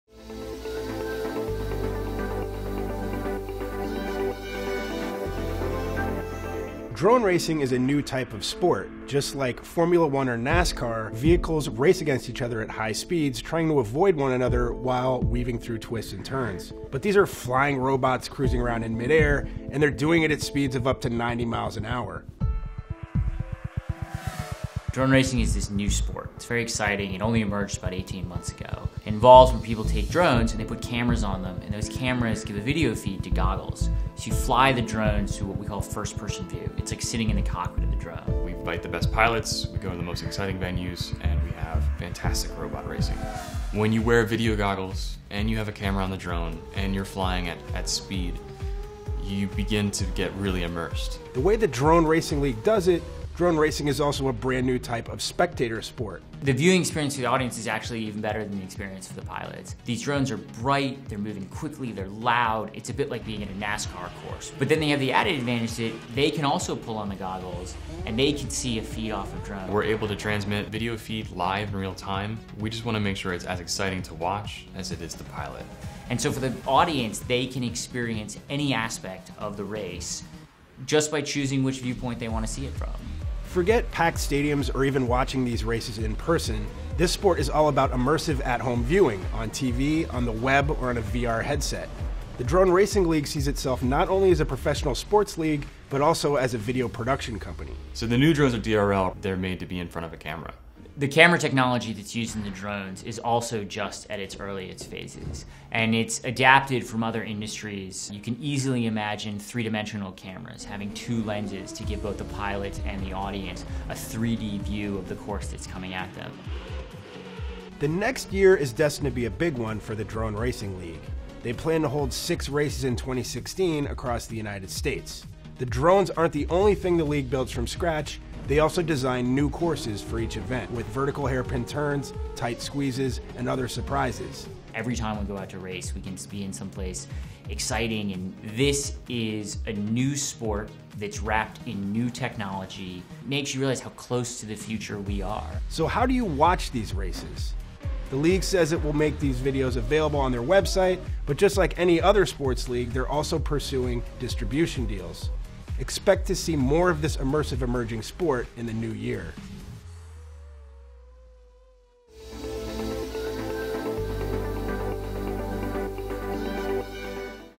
Inside the World of Drone Racing | WIRED
The world of drone racing is becoming an increasingly popular sport. Like a special-effects sequence from a sci-fi movie, little glowing spaceships zip around in tight formations, weaving through neon gates, slithering through tight tunnels and occasionally smashing into walls at high speeds. We speak to the sport's pioneers to find out more about the technology involved